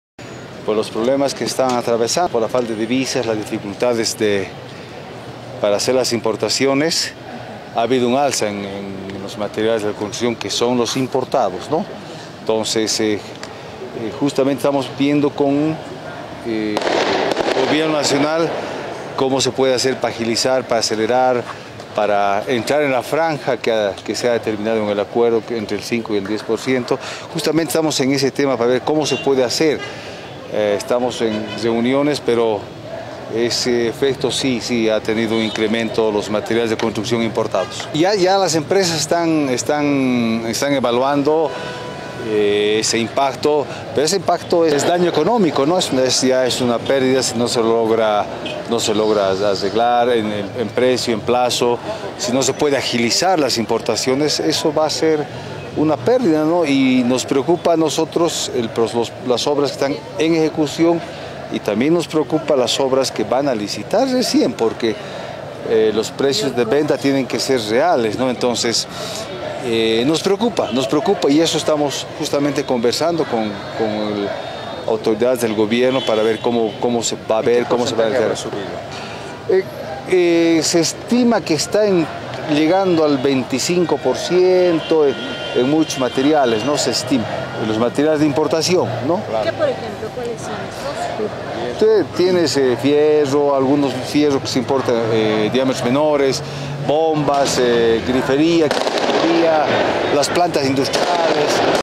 Cámara Boliviana de la Construcción: Denuncia que aun tiene dificultades en consegui
Cámara Boliviana de la Construcción: Denuncia que aun tiene dificultades en conseguir " dólar" que provoca incremento del 25% en importación de insumos.

El Presidente de la Cámara Boliviana de la Construcción Luis Bustillo. Indicó que su sector aún tiene "dificultades" en conseguir dólar en el mercado interno para realizar sus  transacciones en la importación de insumos que se registró un alza en los materiales de construcción que son los importados hasta un 25% el sector de la construcción pide una reunión con el gobierno central para buscar una solución a este tema.